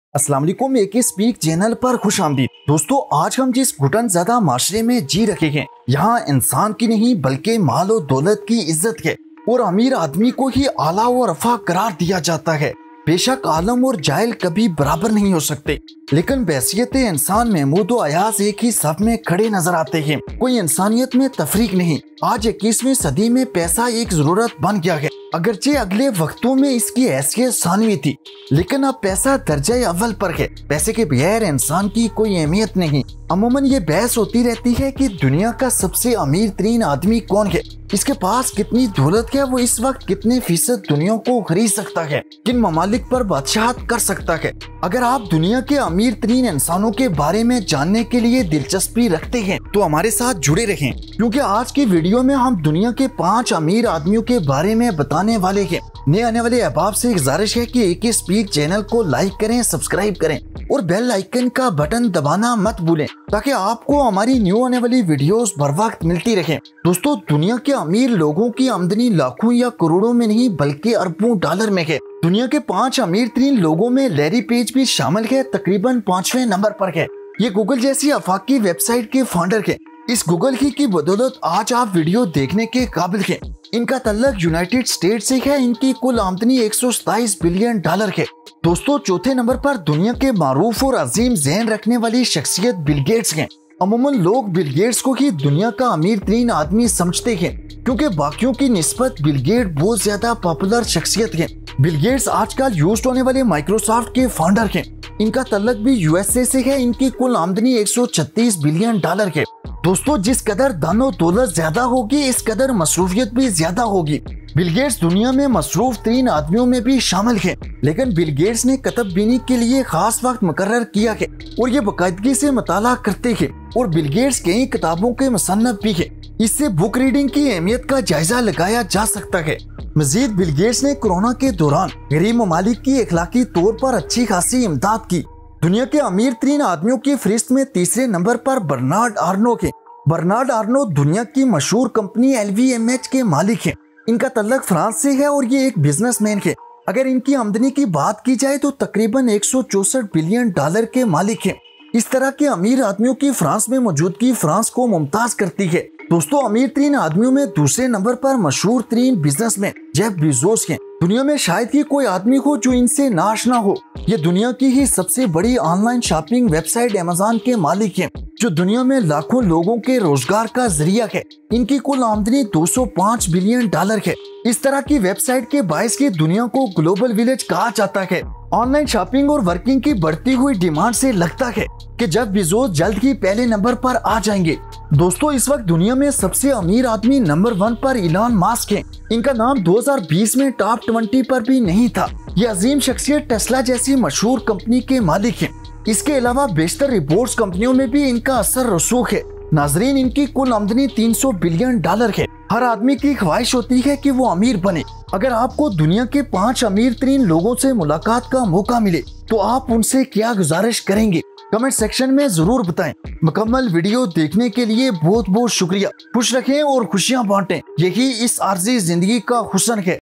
The top five richest persons in the world 2022 | World's Billionaires | Ak Speaks
Name of the top 5 richest people in the world are:

5) Larry's page
4) bill gates
3) Bernard Arnault
2) Jeff Bezos
1) Elon musk

Ak Speaks Ko Follow Karna Na Bolna:-